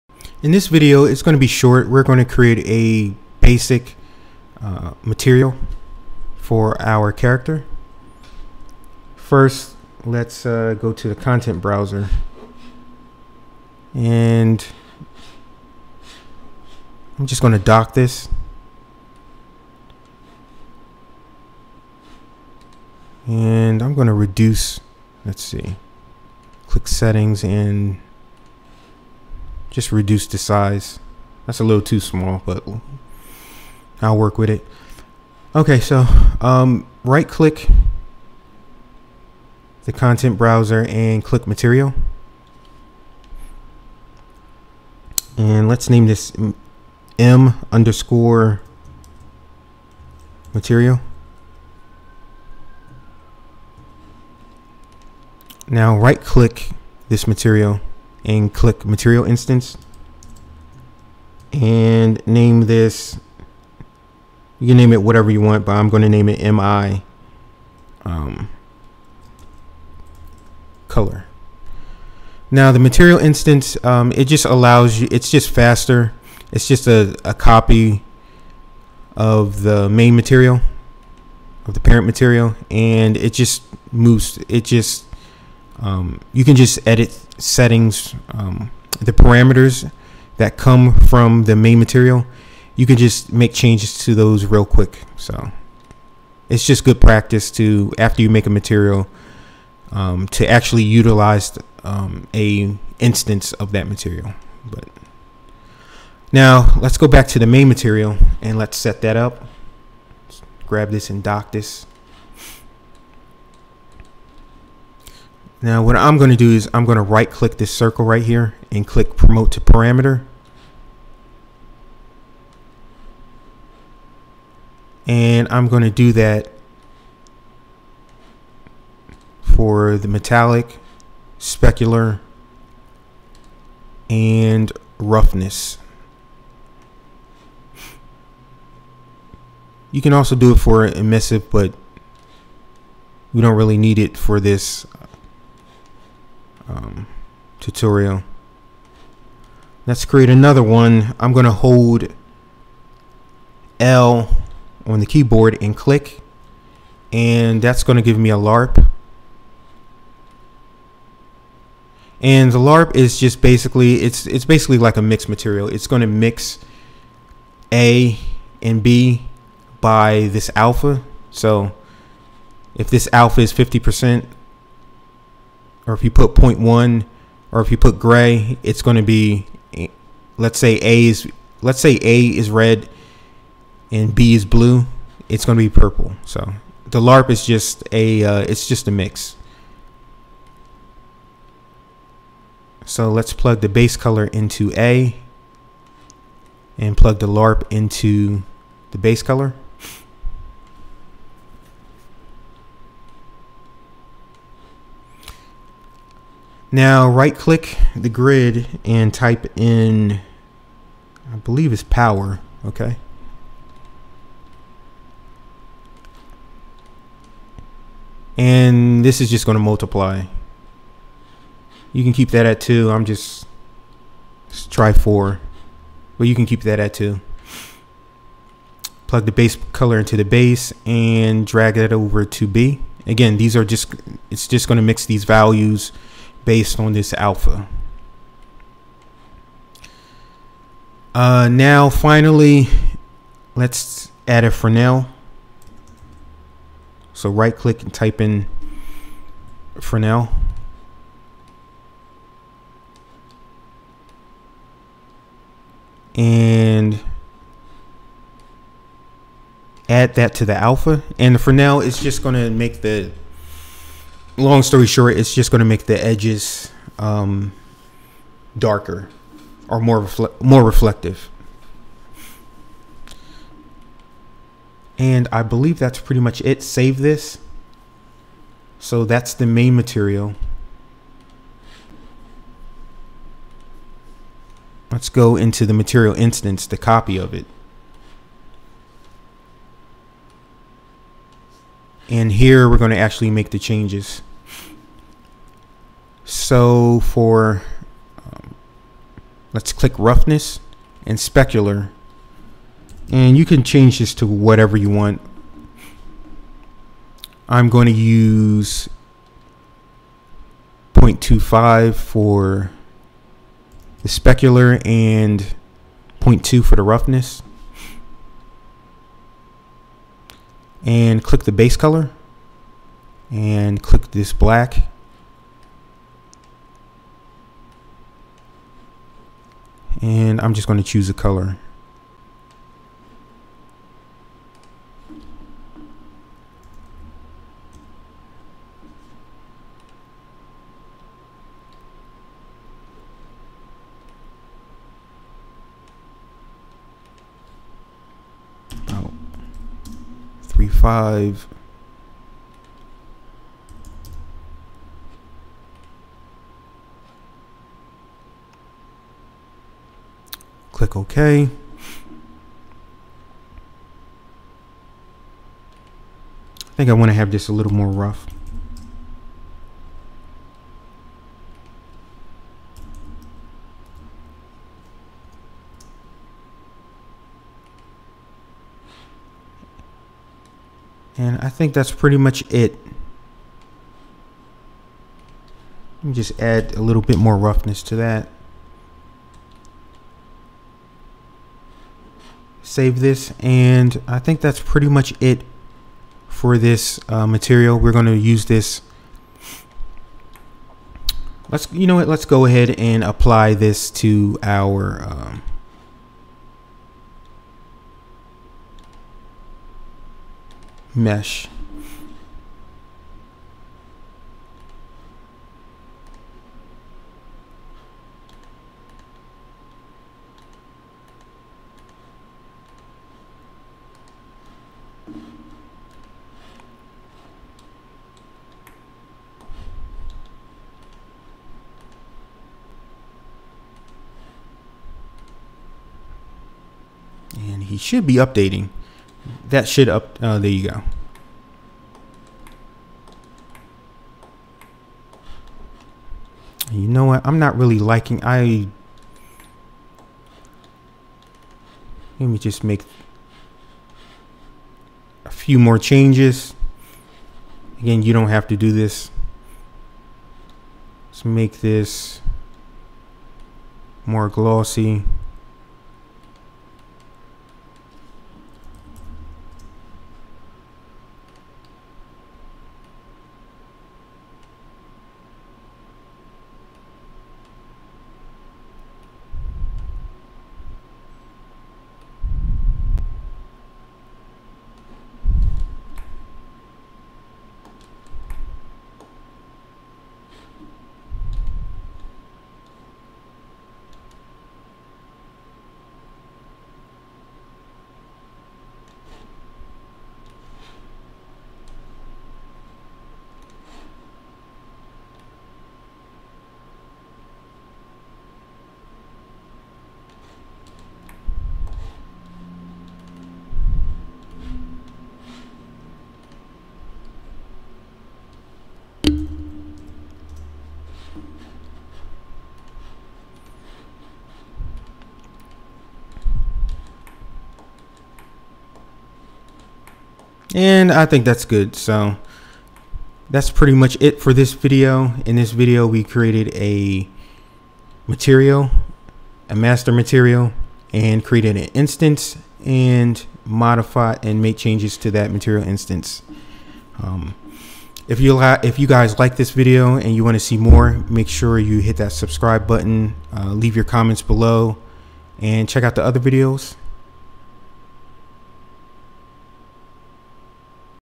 Unreal Engine 5 - How To Create A Third-Person Game | #03 Basic Material for Character
Updated series of videos walking you through the steps to create a third person game in UE5.

00:00 Outline
00:35 Create Material
00:52 Create Material Instance
01:45 Edit Main Material
04:45 Edit Material Instance
06:50 Apply Material
08:50 Recap